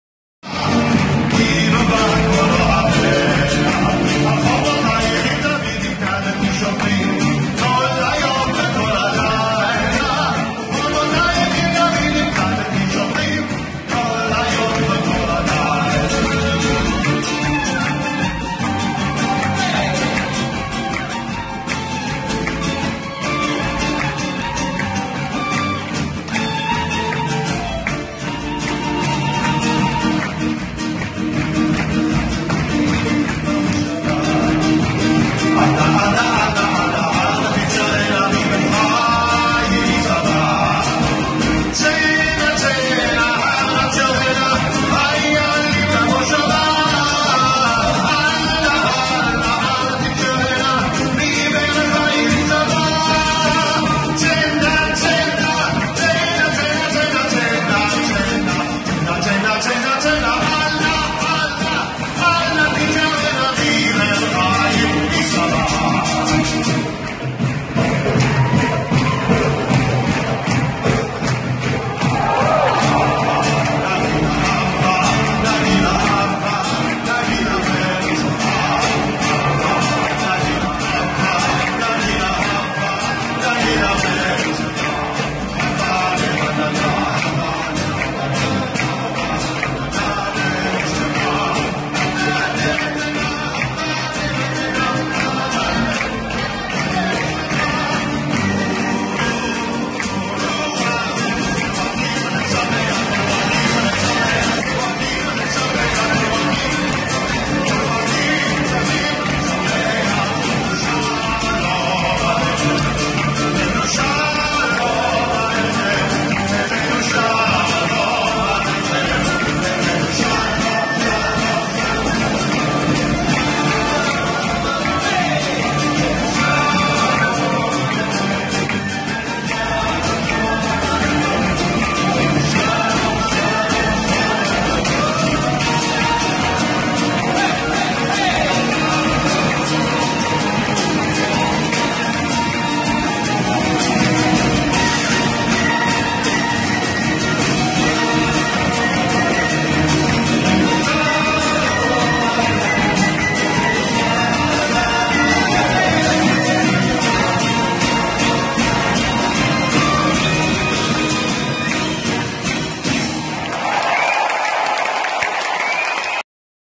Shir - Hora dancing with IDI dancers
Balfour Centenary Event Nov 2017 at The Royal Albert Hall.
With the Israel Dance Institute Dancers directed by Ilai Speziak